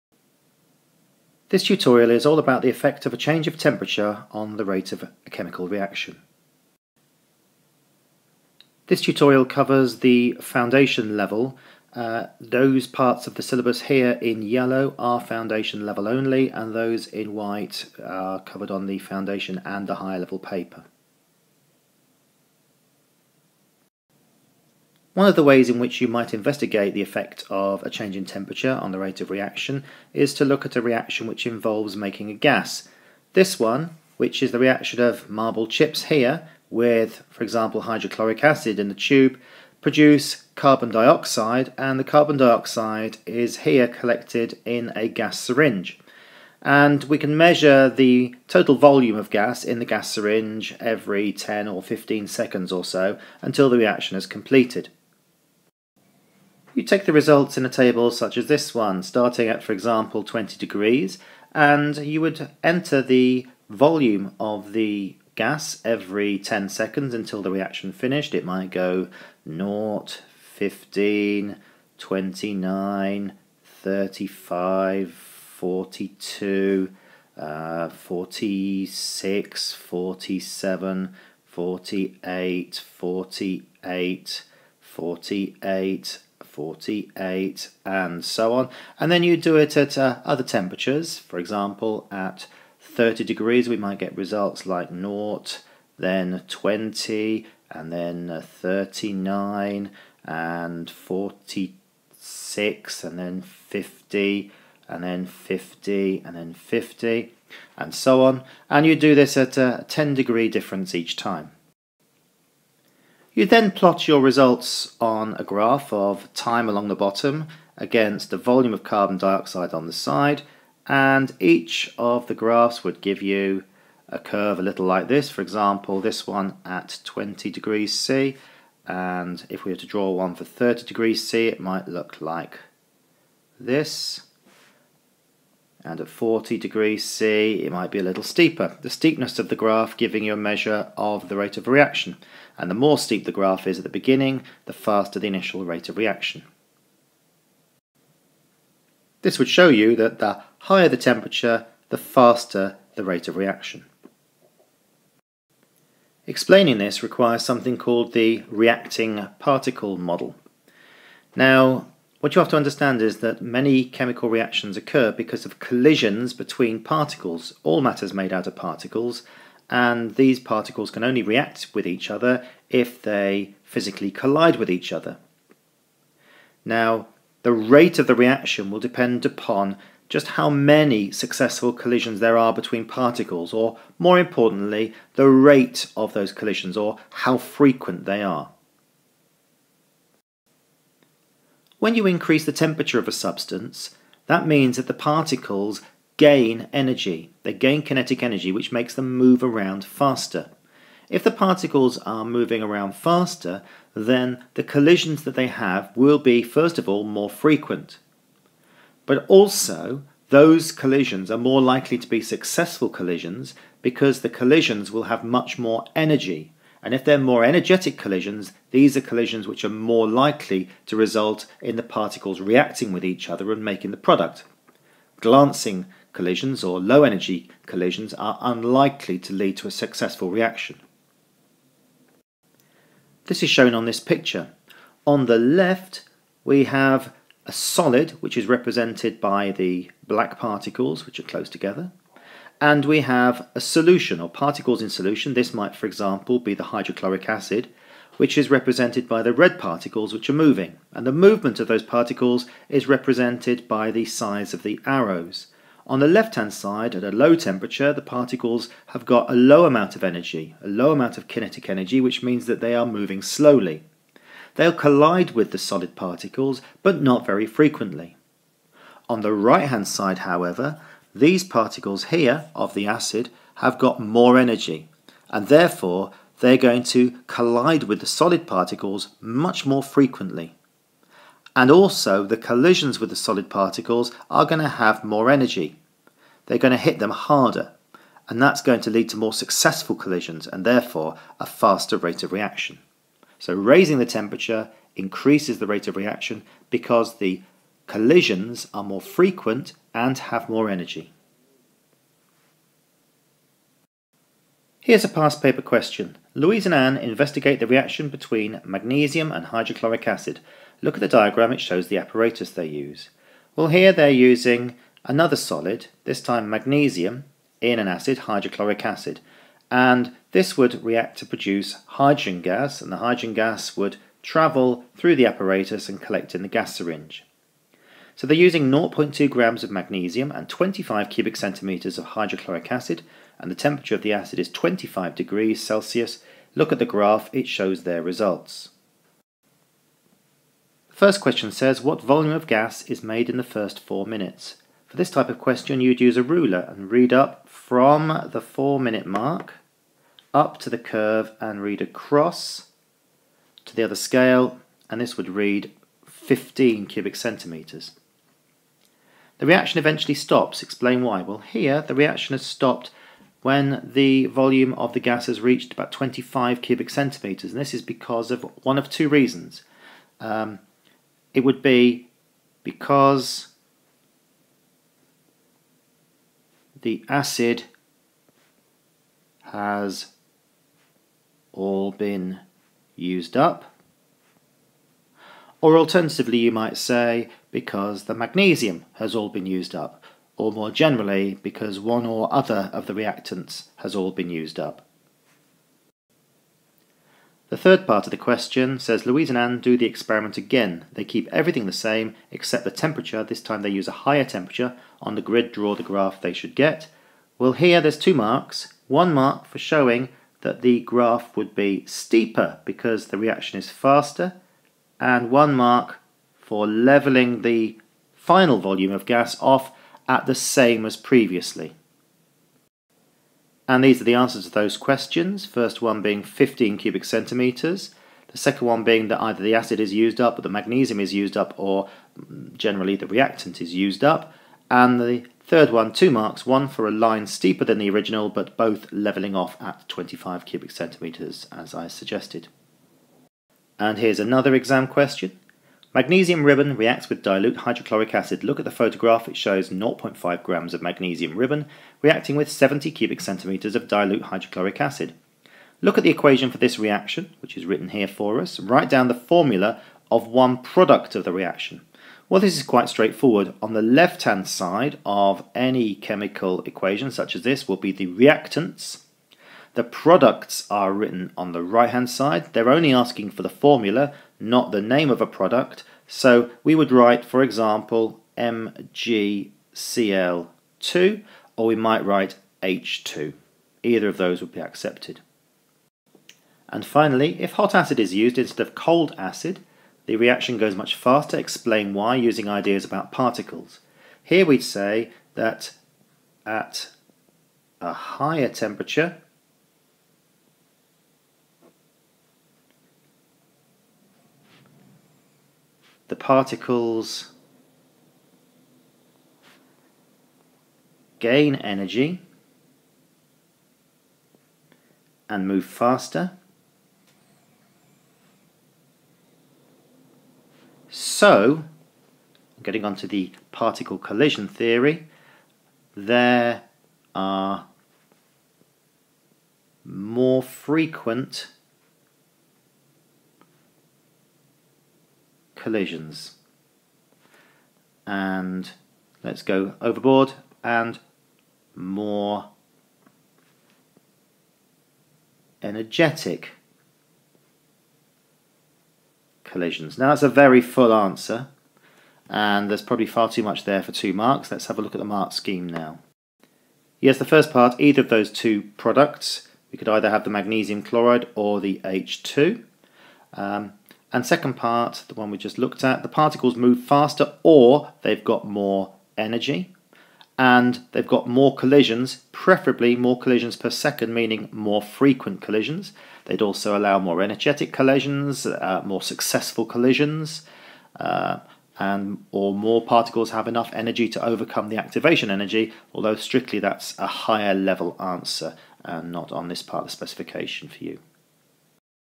OCR Gateway B C3 Reaction Rates - Temperature (Foundation)
Tutorial covering the effect of a change in temperature on the rate of reaction, including an explanation using the reacting particles model.